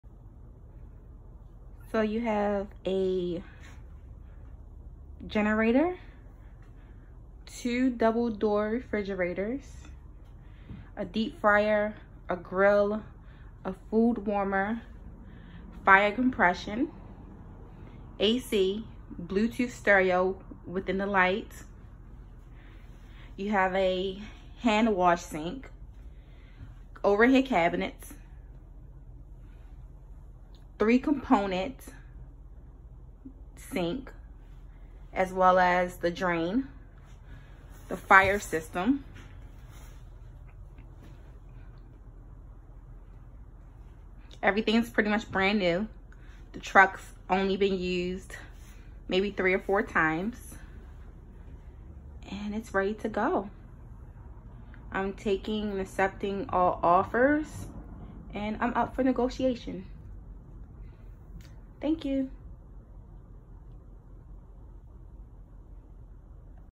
Maryland Fully Loaded Food Trailer for Sale
Fully loaded Food trailer for sale.

2018 - 6' x 14' Lightly Used Snapper Food Concession Trailer for Sale in Maryland